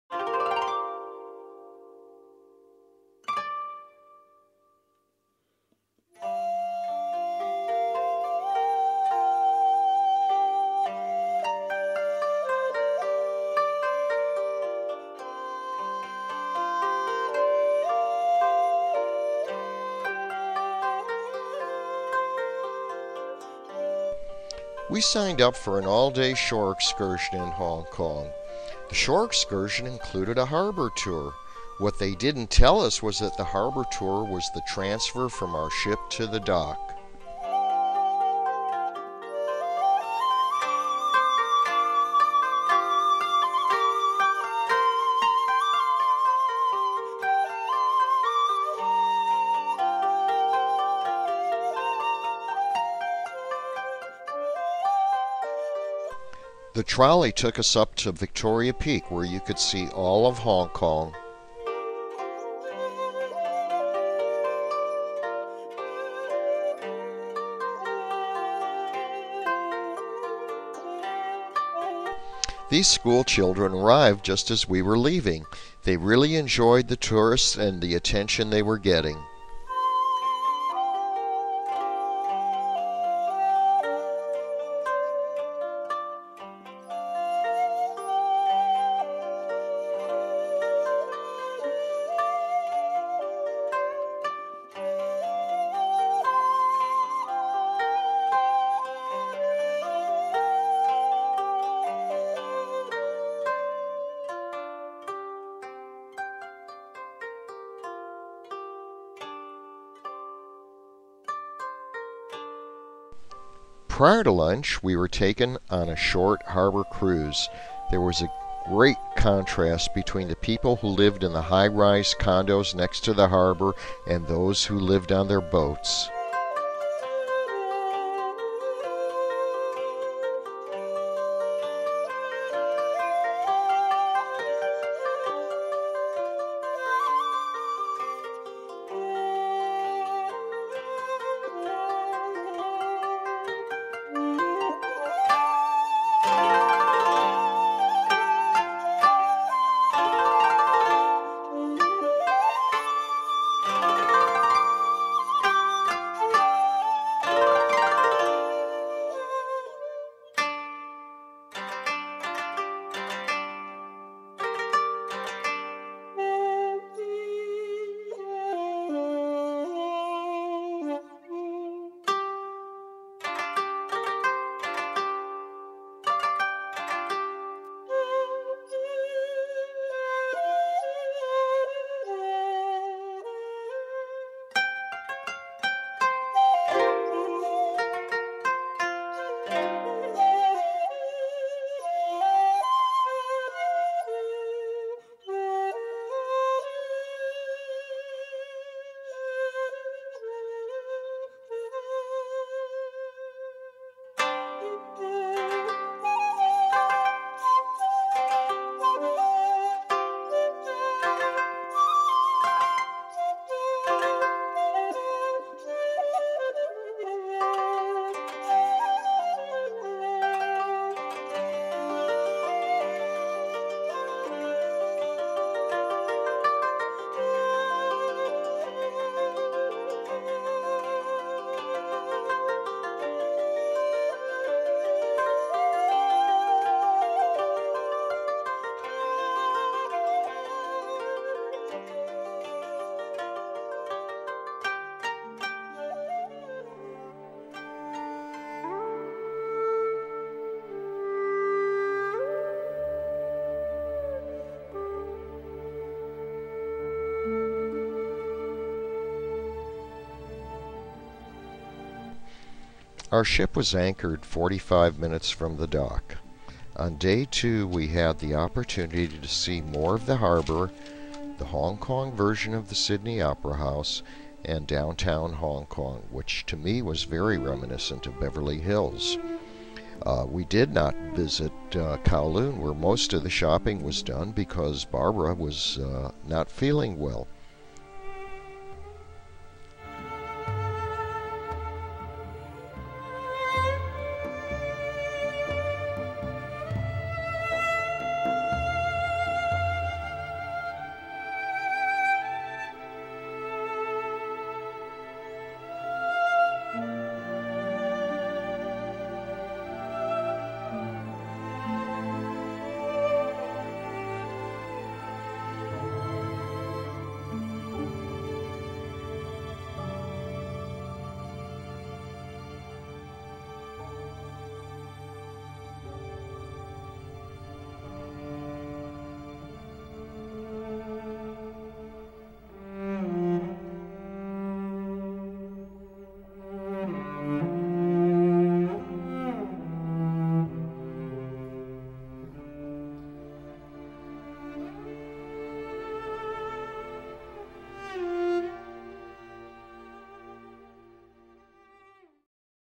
Hong Kong, China
Our cruise ship stayed overnight in Hong Kong harbor.  The first day we took a shore excursion and the next day Barbara and I walked around the city.